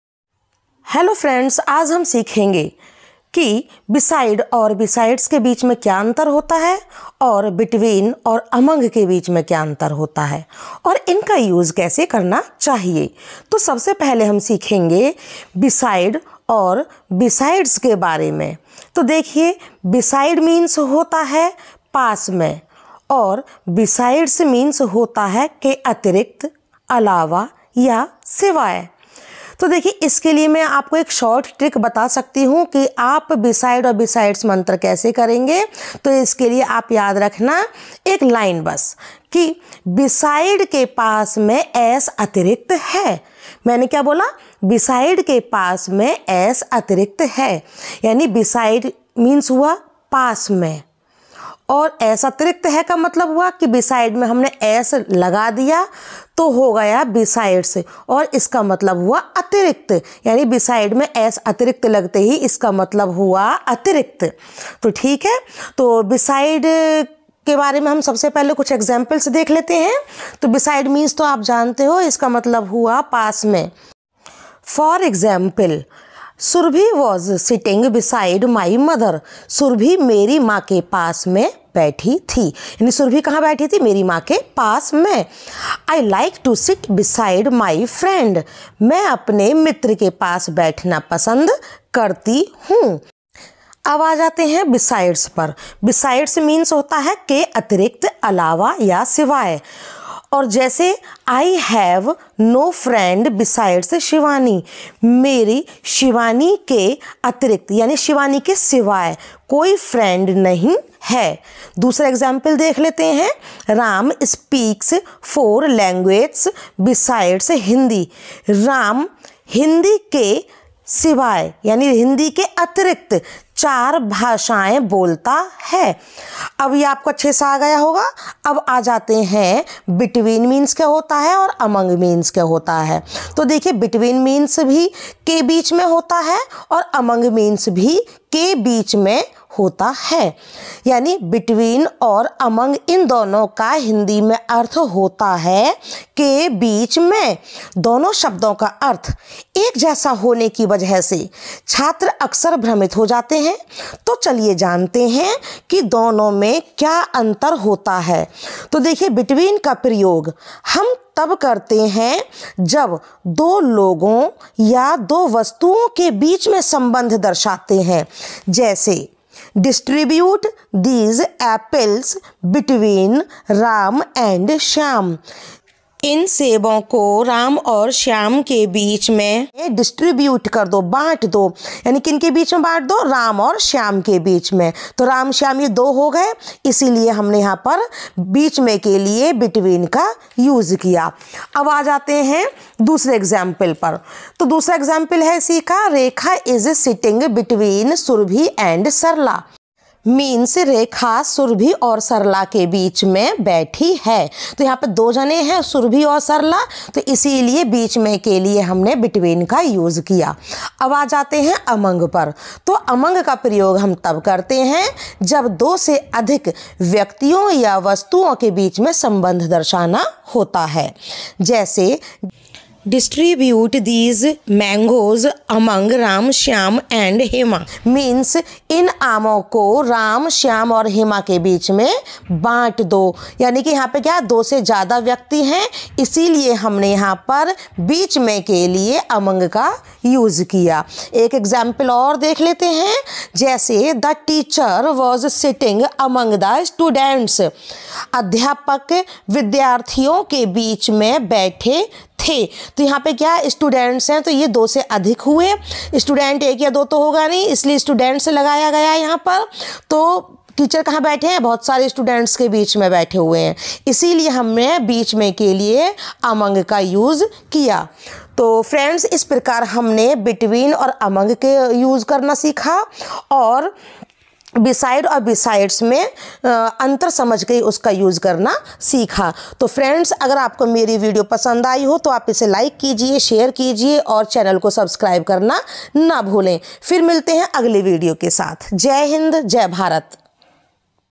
use of beside and besides | between and among
All the videos of our channel are educational.

In this video I will tell you what is the difference between beside and besides, and between and among.